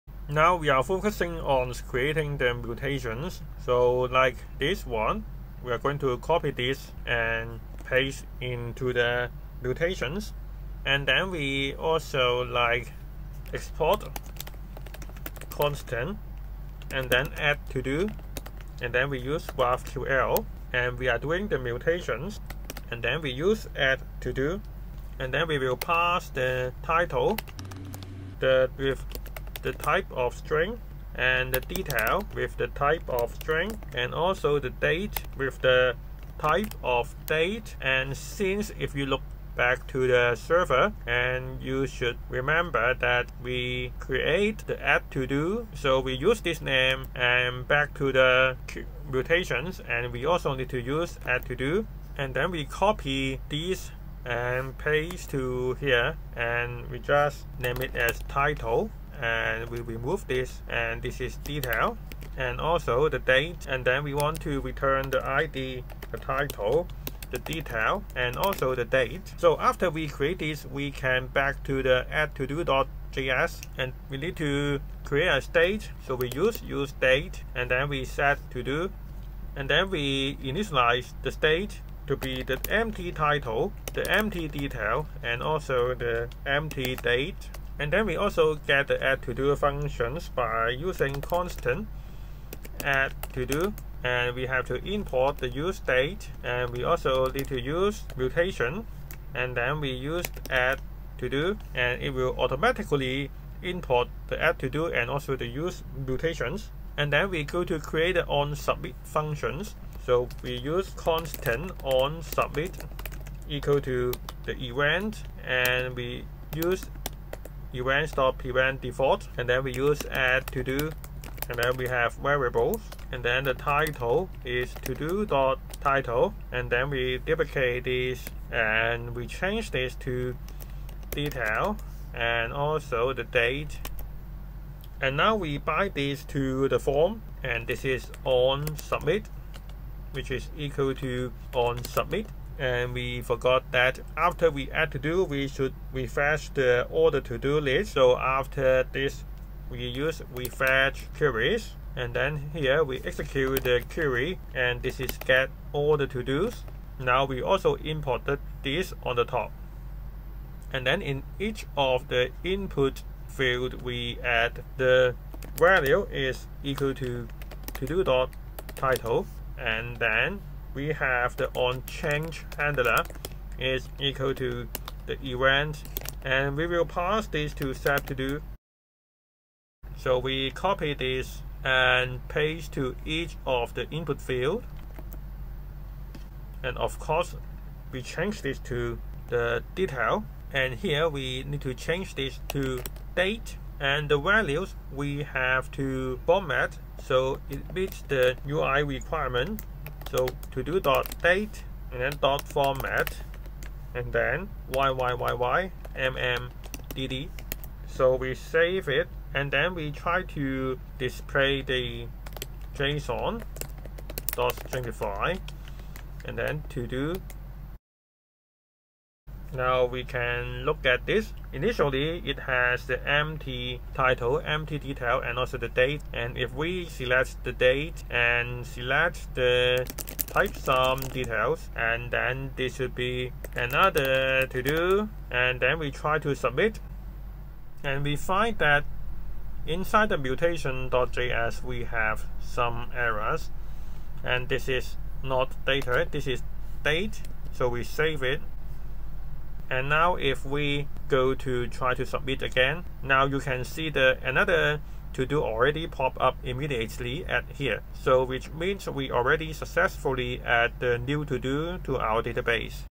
React, GraphQL, MongoDB and Apollo Client | Create Todo App #8 Create a new todo
In  this part, we are going to use the useMutation to create addtodo function, after create a new todo, we will use refetch queries to get all the updated todo list.

🔥Complete Courses🔥
Gumroad Web and mobile complete courses

Macbook Pro

Camtasia

webdevelopement react tutorial